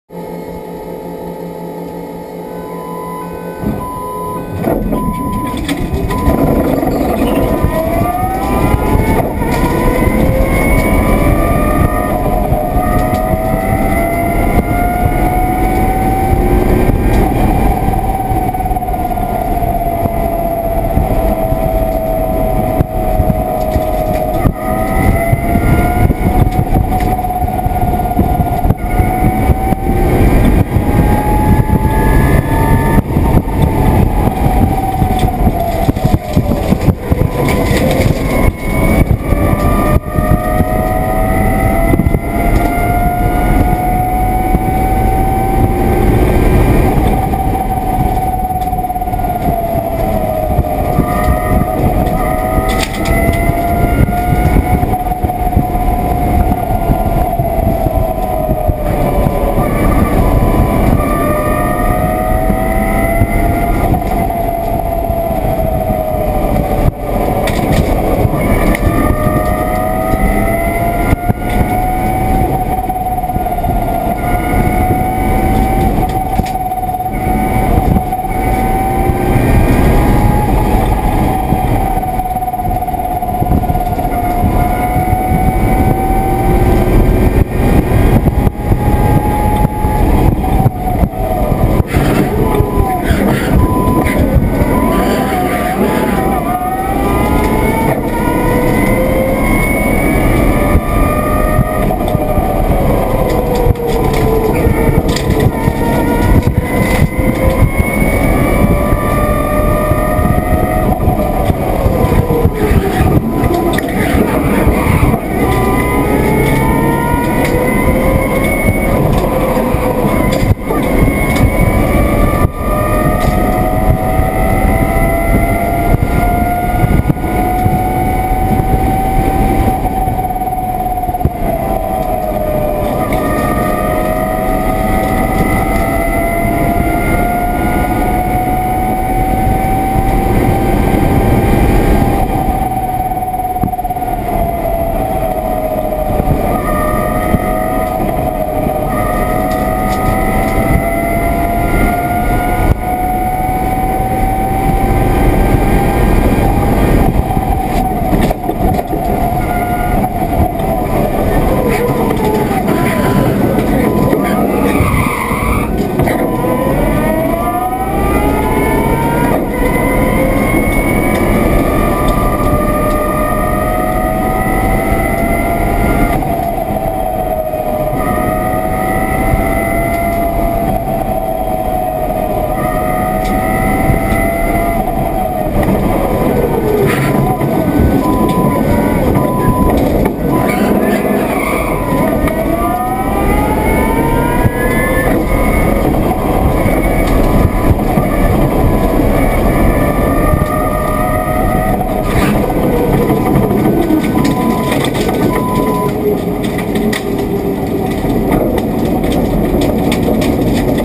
EV West Pikes Peak 2012 - Top Section Practice Run Electric M3
Here's the practice run from Devil's Playground up to the summit of Pikes Peak at 14,140 feet. The car was running strong, but we kept it in the safe zone as the section is known for cars going off. It feels good to reach the summit in an electric car with no loss of power due to the thin air. Actually, the only loss of power was to our team which resides at sea level and wasn't totally acclimated to the high altitude.